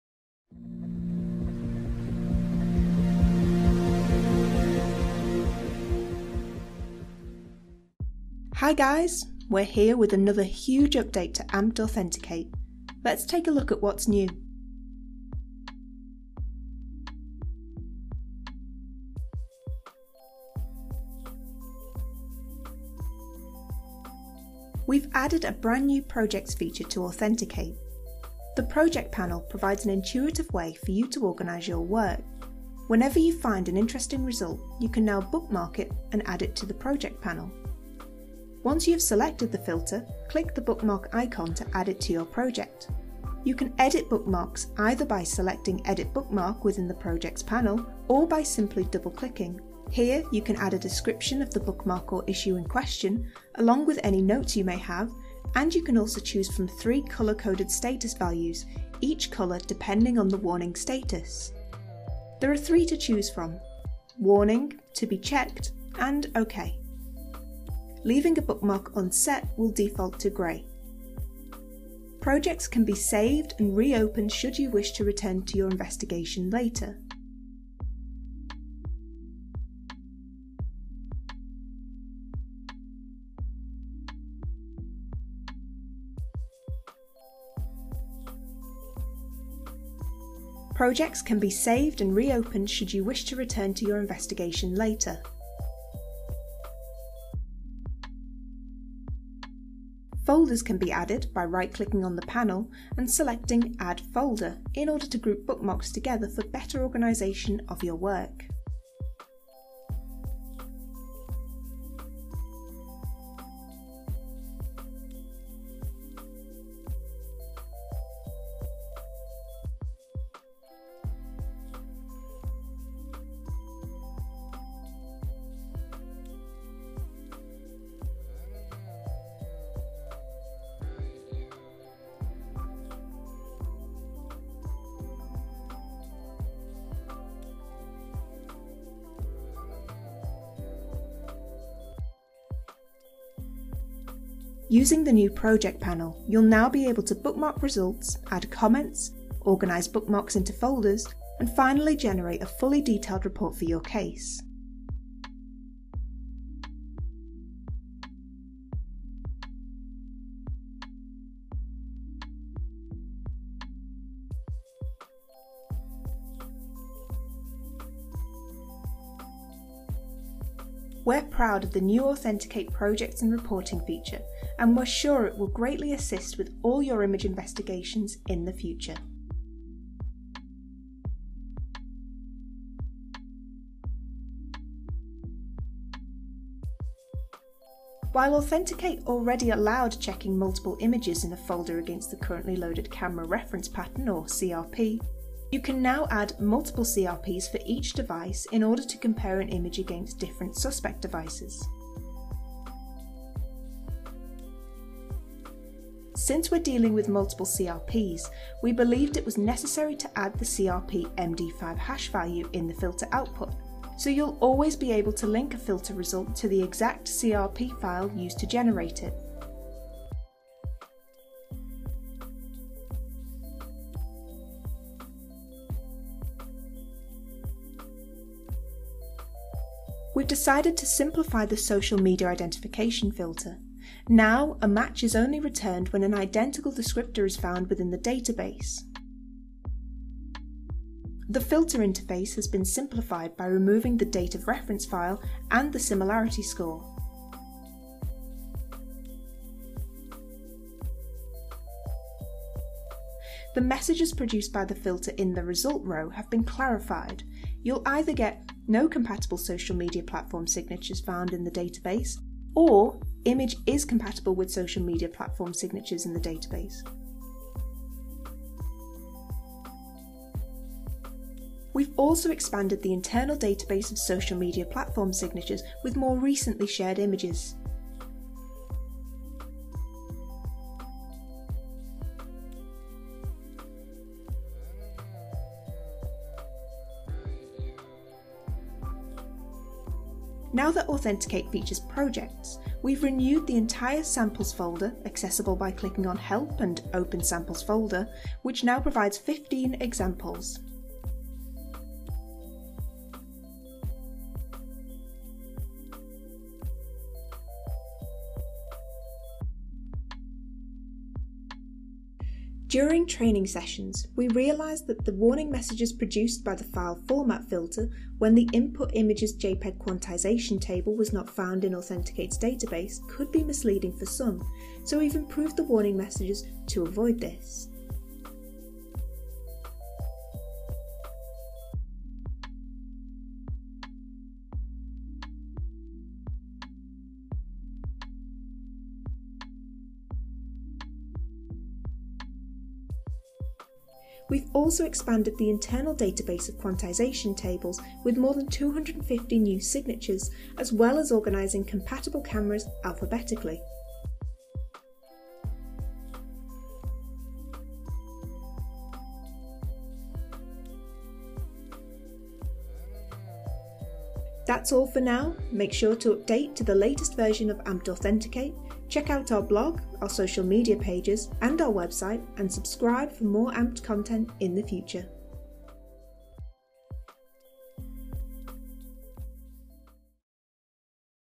Amped Authenticate Update 13901: Projects, Reporting, Improved Social Media Identification and More
In this release we've added a brand new Projects and Reporting feature, PRNU with multiple reference cameras, improved Social Media Identification, and much more